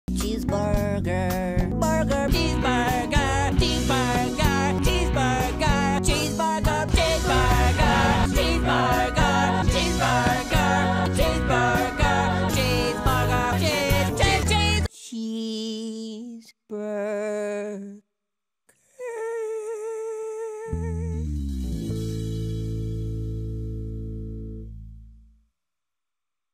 his cheeseburger but only "cheeseburger," "cheese," and "burger"
i keep telling myself, "the next video i upload will be high-effort," and it keeps not being the case